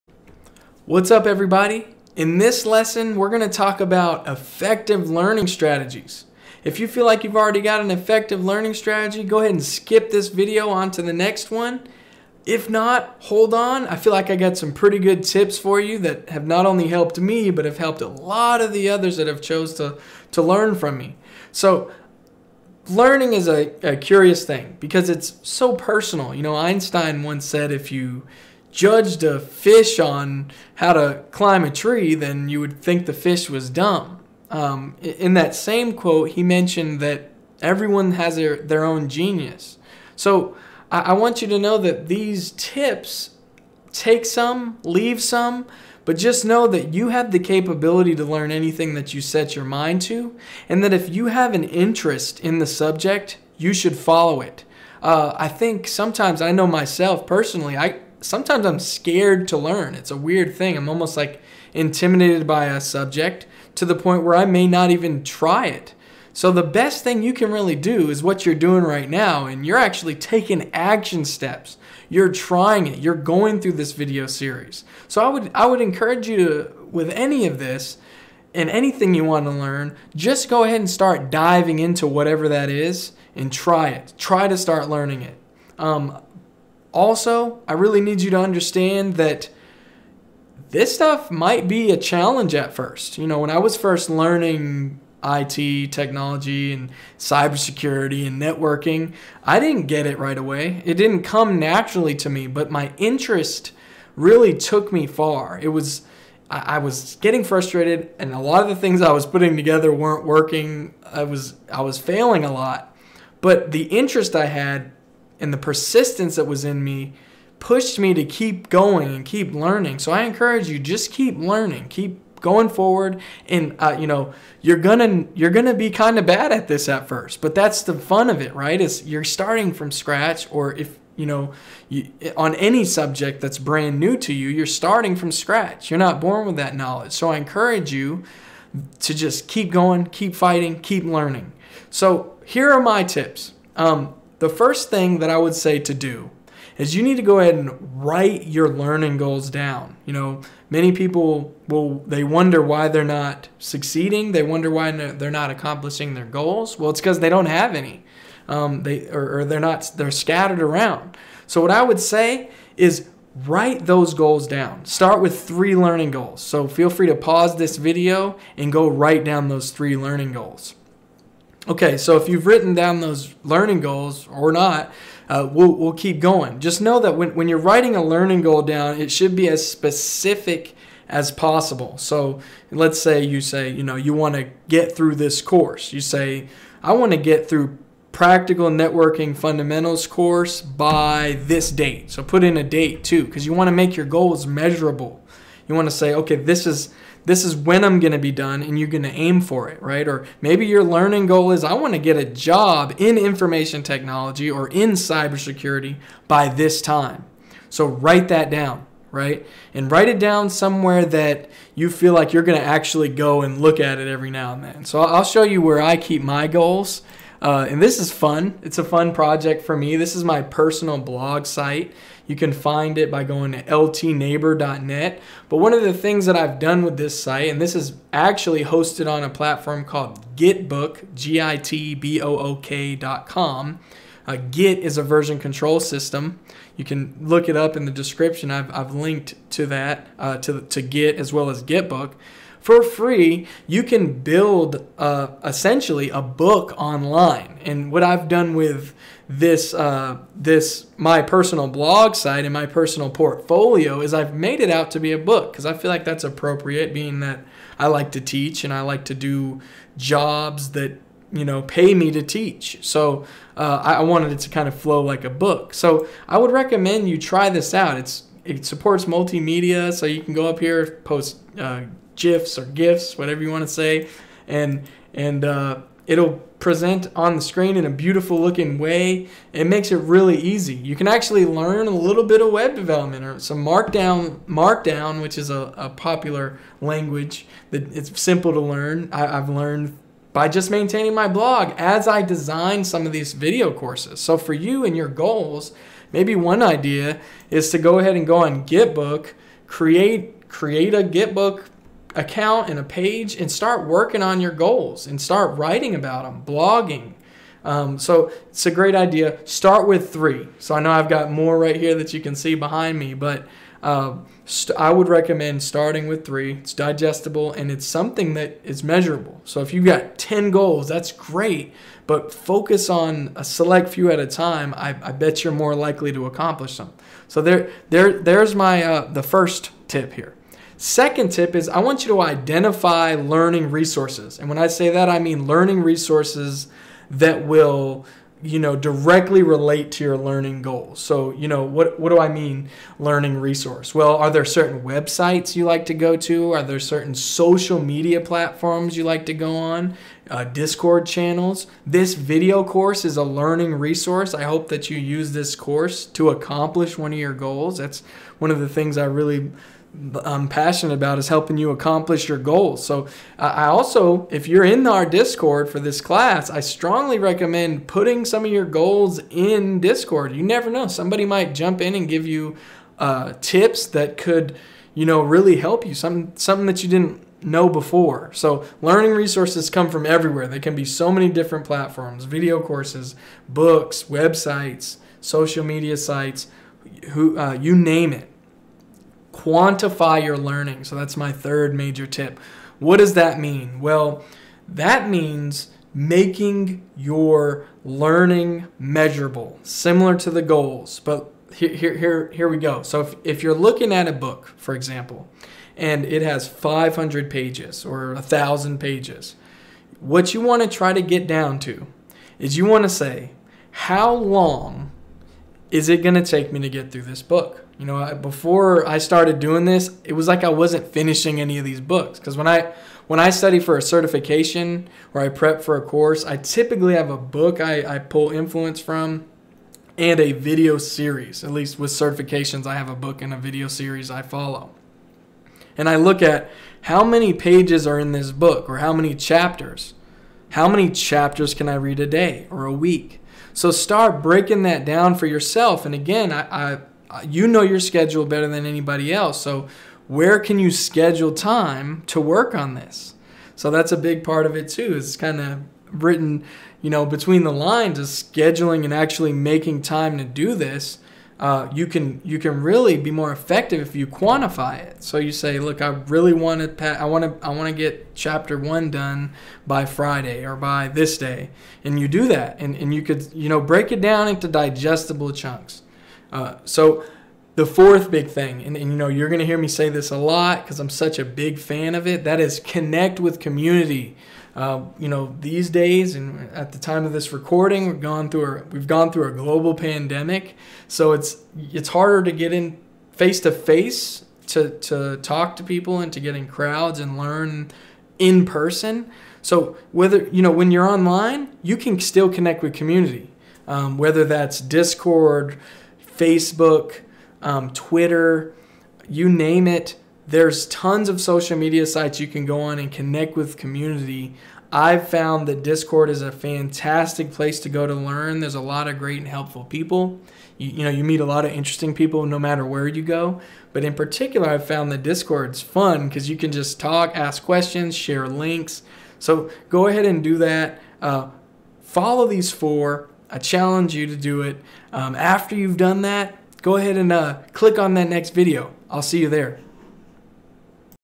Tips for Effective Learning Strategies in IT & Cybersecurity
In the second lesson of the Practical Networking Fundamentals course we discuss how you can use the power of social media to amplify your learning process not matter what skills you are trying to develop.

Please know that this course is not designed to help you pass a test but to help you build applicable skills. The knowledge you gain from this course can help you start to prepare for the following industry recognized certifications:

- CCNA
- Comptia A+
- Comptia Net+
- Comptia Security +
- HTB CPTS
- OSCP

Course Instructor:
Robert "Bob" Theisen

Learning Objectives Covered:

- Learning strategies for IT & Cybersecurity
- Using LinkedIn to learn
- Quantifying your learning process
- Collaborating with others to help guide your learning
- Developing learning goals
- Understanding IT & Cybersecurity industry trends
- Using Discord to learn
- Creating a personal blog to demonstrate skills